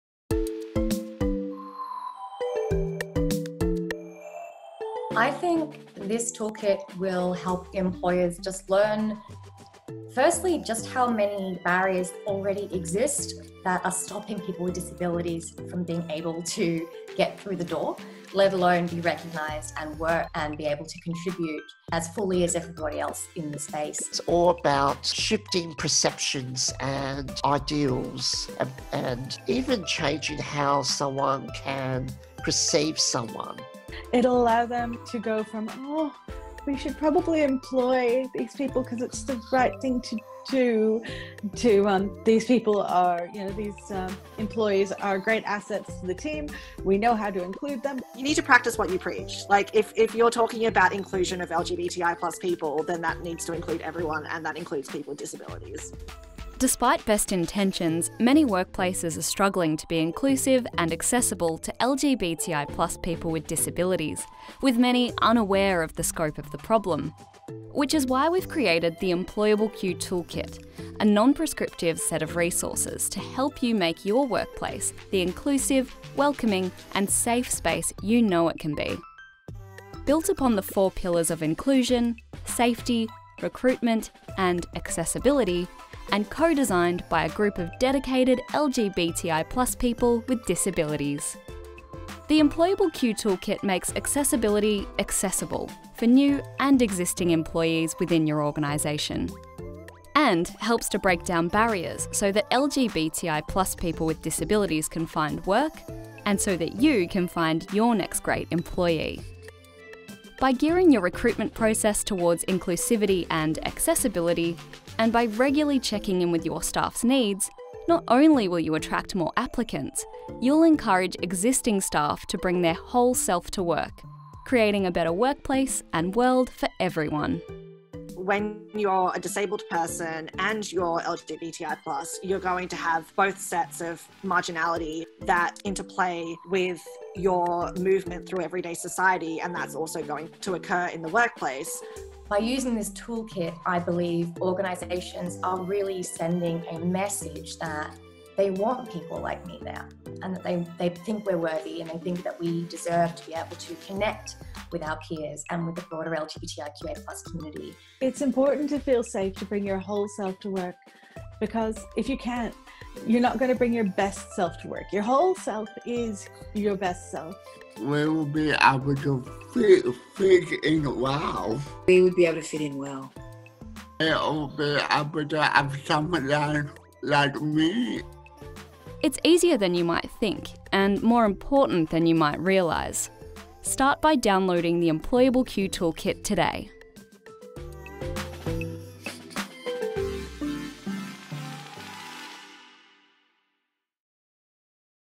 Developing the EmployableQ Disability Inclusion Toolkit
The EmployableQ toolkit provides a non-prescriptive set of resources to guide and support organisations to build inclusive, welcoming, and safe workplaces for LGBTI+ people with disability, underpinned by the 4 Pillars of Inclusion.